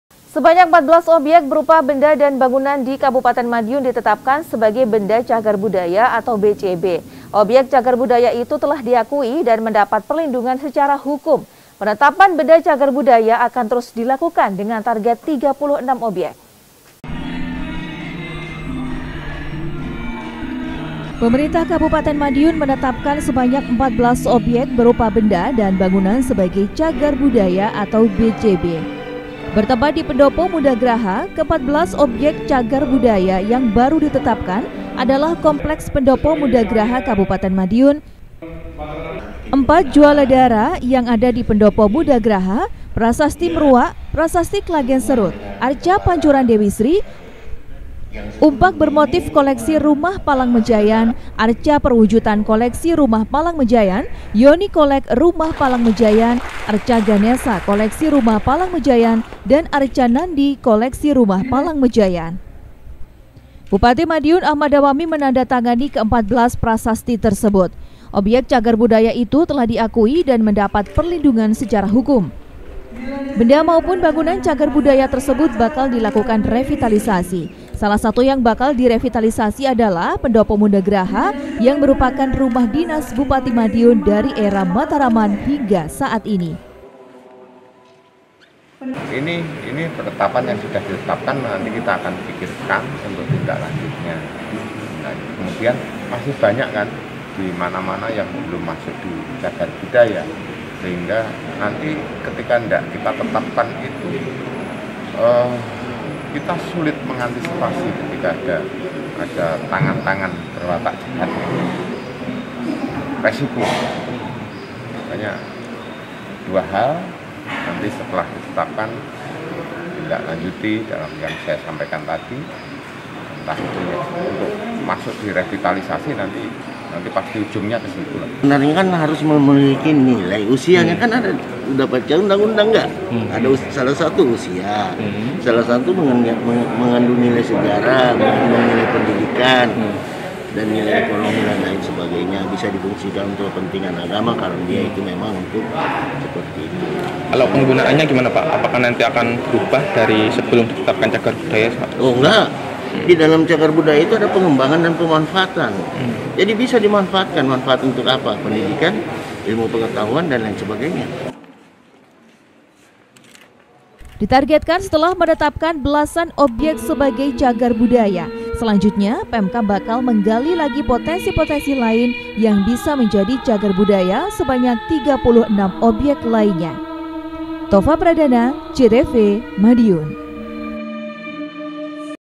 KAB MADIUN - 14 Objek Bersejarah Ditetapkan Sebagai Benda Cagar Budaya
Sebanyak  14 objek berupa benda dan bangunan di kabupaten Madiun ditetapkan sebagai Benda Cagar Budaya atau BCB.  Objek cagar budaya itu telah diakui dan mendapat perlindungan secara hukum.  Penetapan benda cagar budaya akan terus dilakukan dengan target 36 objek.